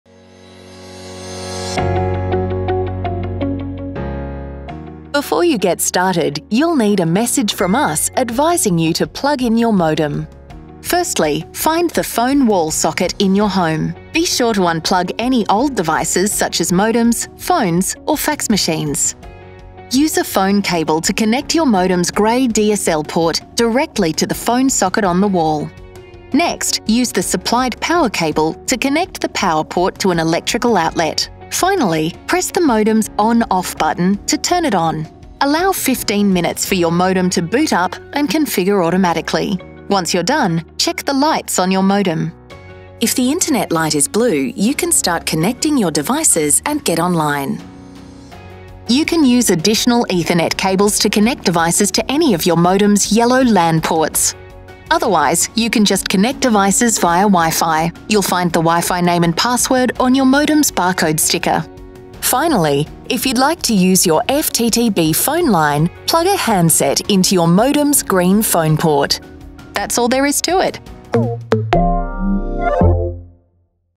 Set up your TPG FTTB with a VX220-G2V Modem
How to plug in your TPG VX220-G2V Modem for TPG FTTB.